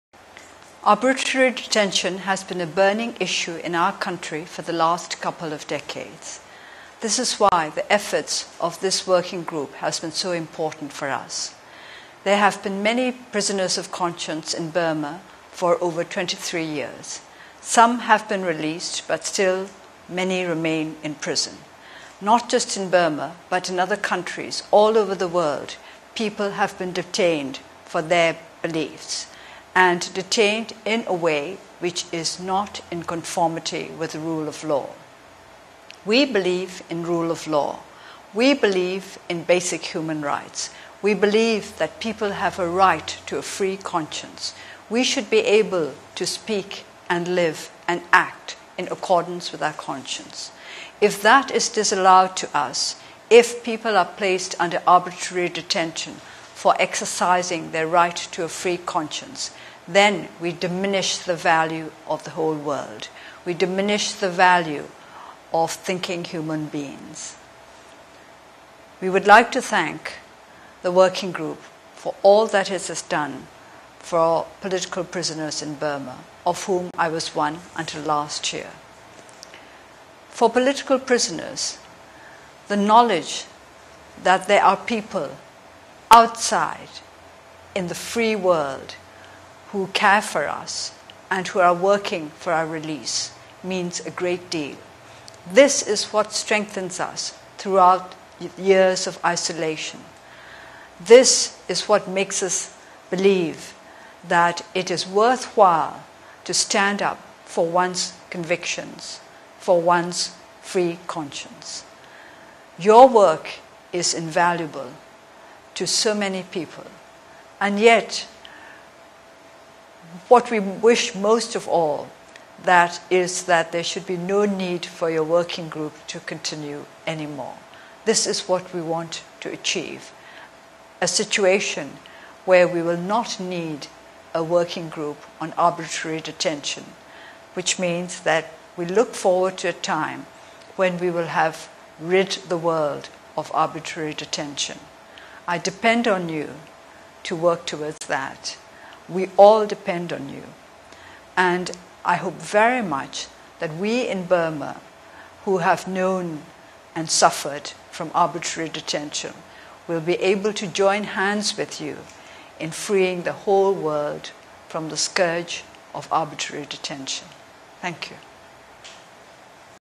"We believe in basic Human Rights" - Message by Aung San Suu Kyi
"If people are placed under arbitrary detention for exercising their rights to a free conscience, then we diminish the value of the whole world. We diminish the value of thinking human beings."

The Working Group on Arbitrary Detention celebrates the 20th anniversary of its creation in November 2011. In this message, Aung San Suu Kyi thanks the Group for all what has been done for political prisoners in the world, especially in Burma. For captives, the knowledge that, somewhere, people are working for their release is a great help. It makes them believe it is worthwhile to stand up for ideas and free conscience.

Instragram : @francediplo